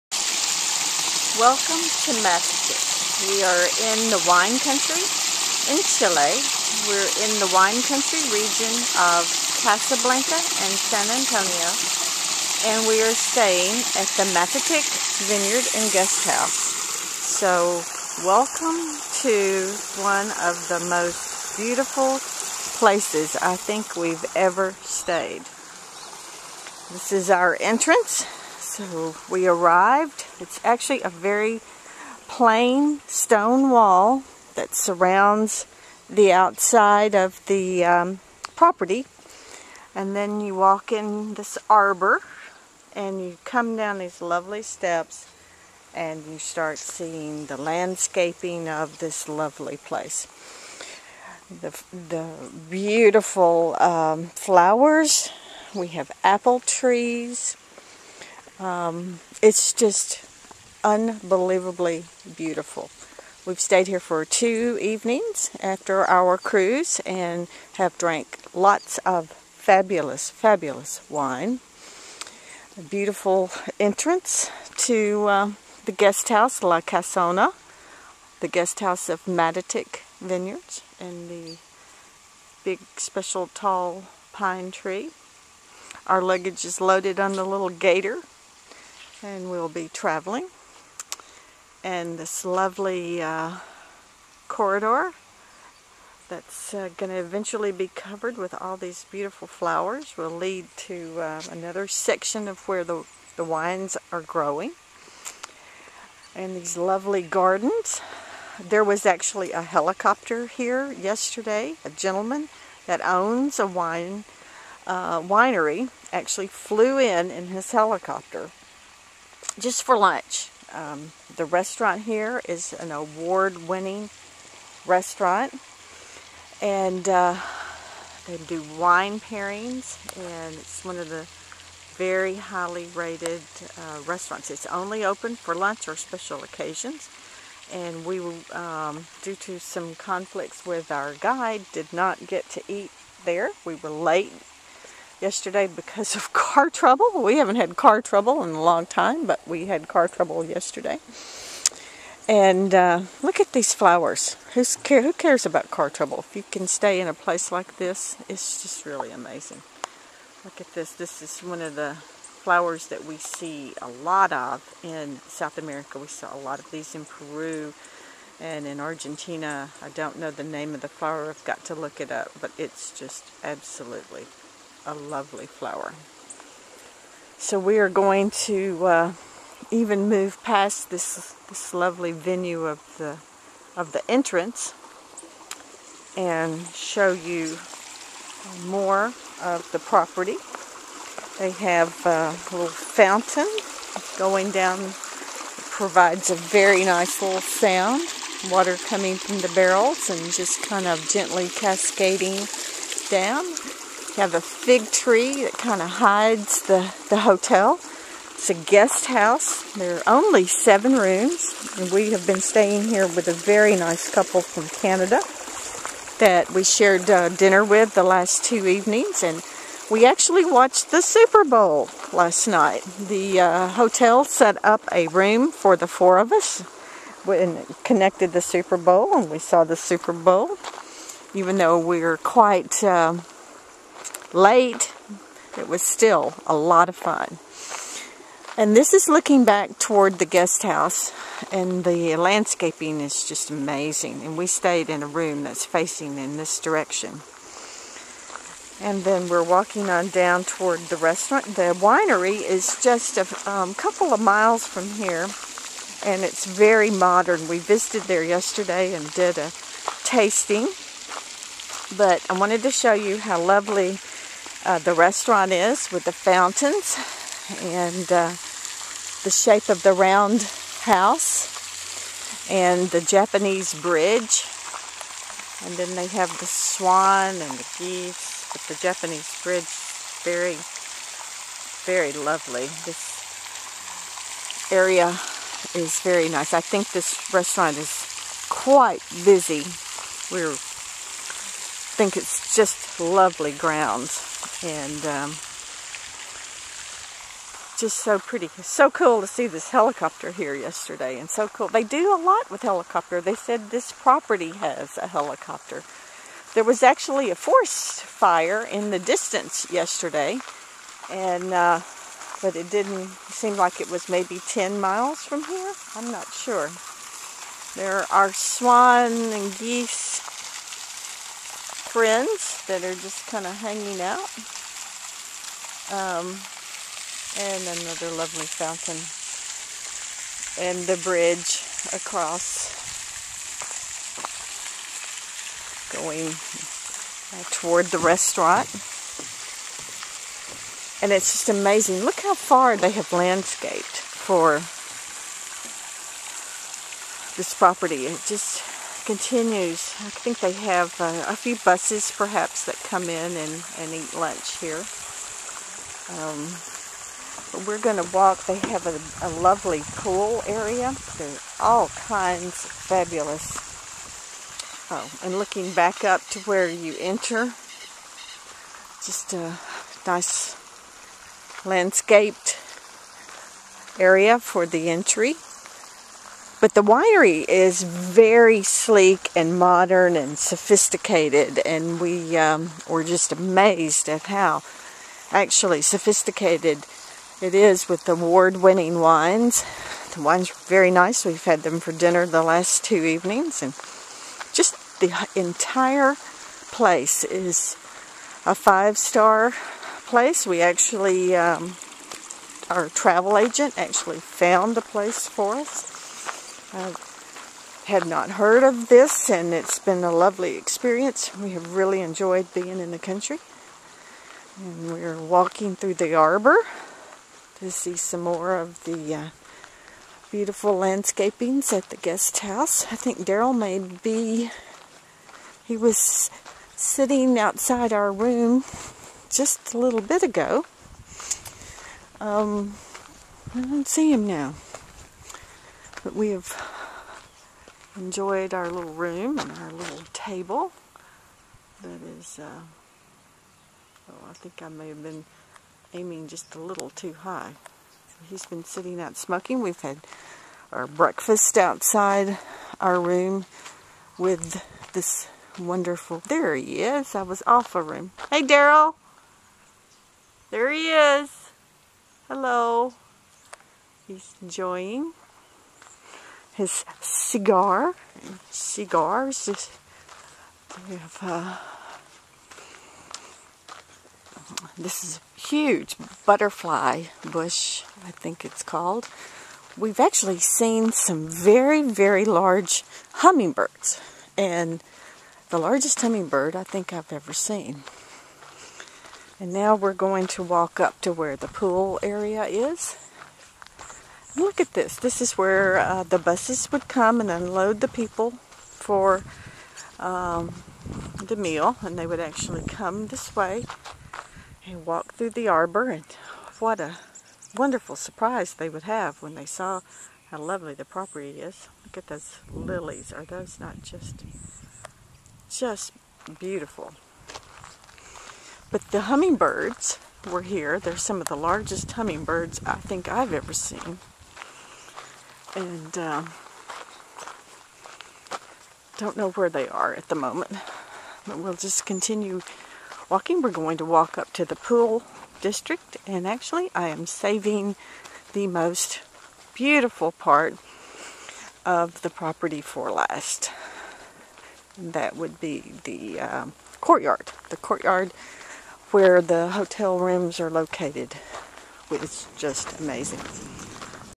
2011cMatetic Vineyards, Chile Part 1
Recorded on February 7, 2011 using a Flip Video camera.  Such an amazing lodging experience with exquisite food and wine pairings!

Also - the Casa opened up a room for several of us to watch the Super Bowl - announced in spanish - but it was fun!!